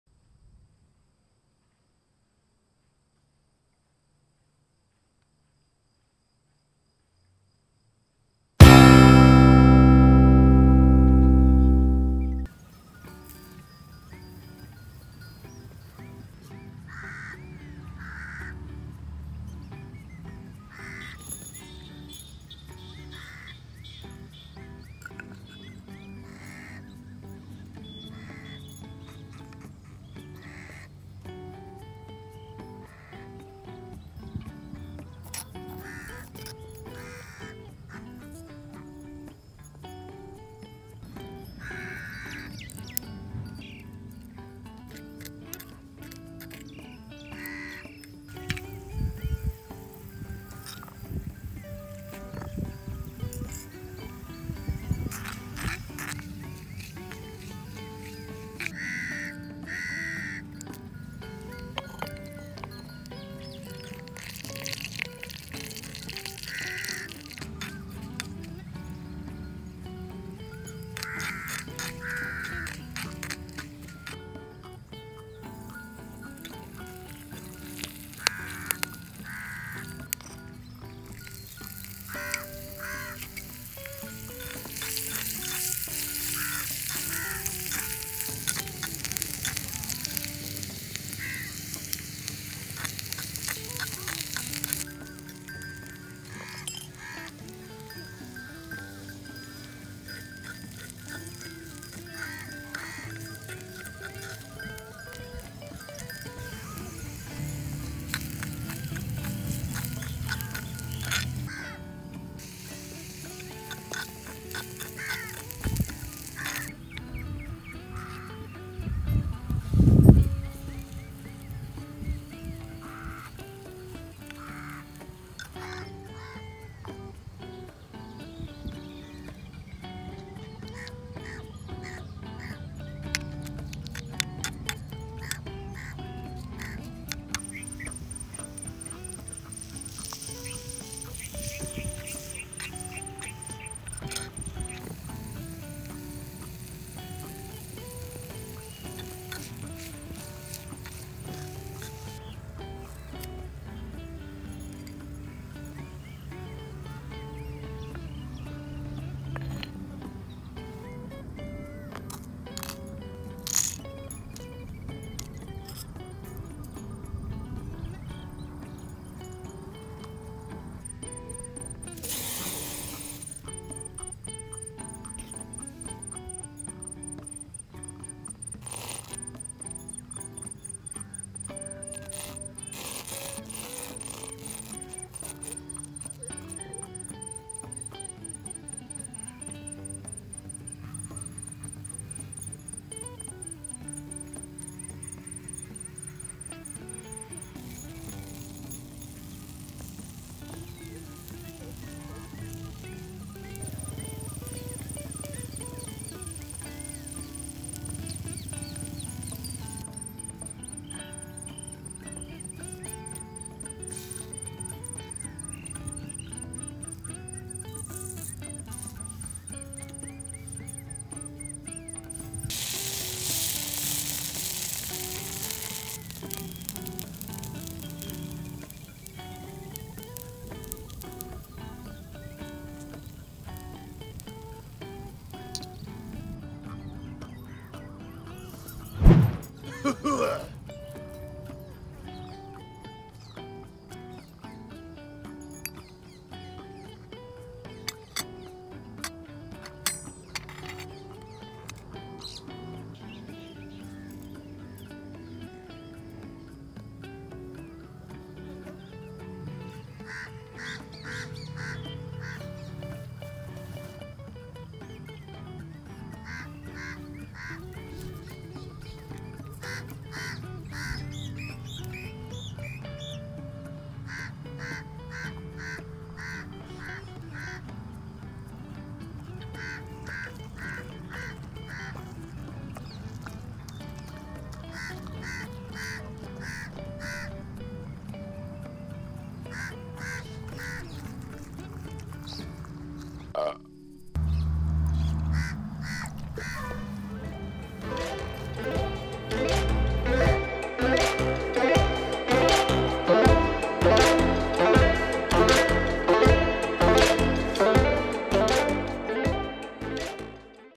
Mysore Masal Dosa Recipe | E#100 | Miniature Dosa Recipe | Miniature Cooking
Miniature Masala Dosa or miniature masala roast recipe with try color chutney. Masala dosa or masala roast is a very popular recipe in south India and masala dosa recipe is a very famous recipe in all over. We prepared this miniature masala dosa recipe with miniature cooking utensils hope you like this recipe.

Miniature cooking dosa | Mini food dosa | Miniature masala roast recipe | Tiny masala dosa | Mini food dosa recipe.